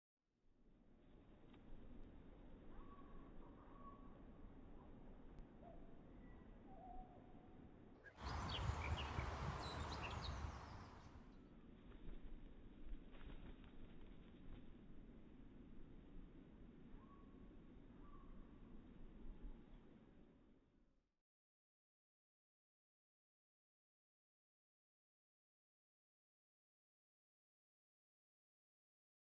Hand Feeding a Robin
While sitting and having lunch, we came across this very confident robin, who ate most of our lovely fruit cake!